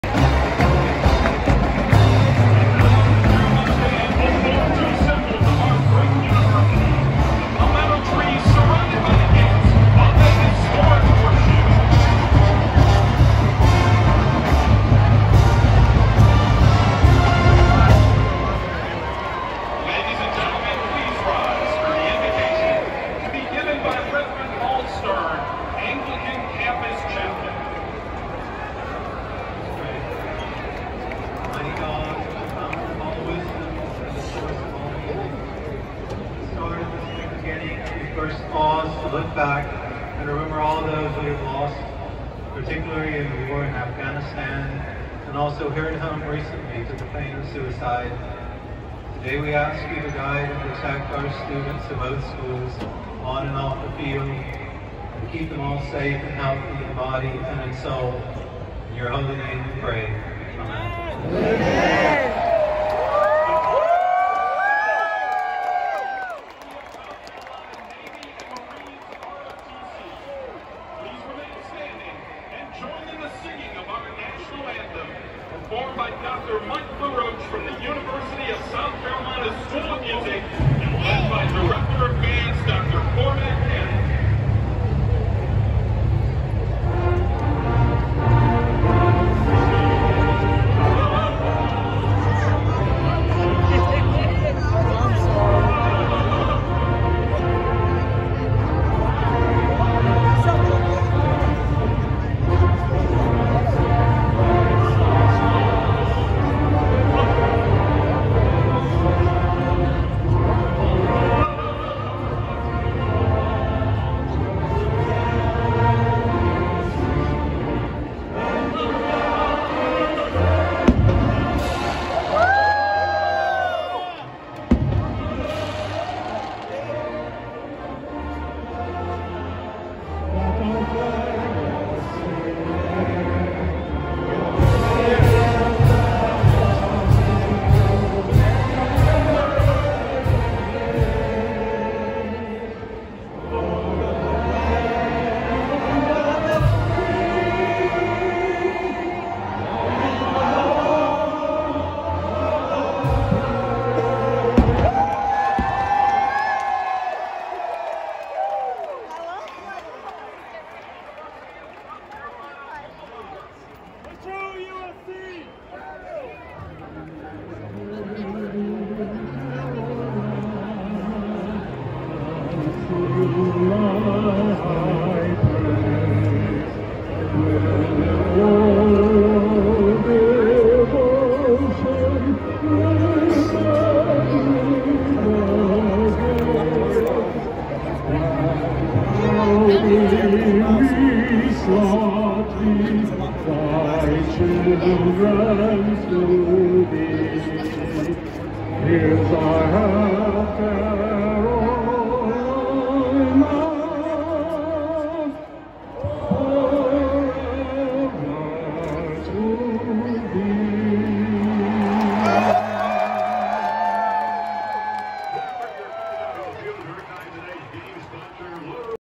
First Shane Beamer Game | U of SC
First Shane Beamer Game at U of SC against Eastern Illinois opening with band etc.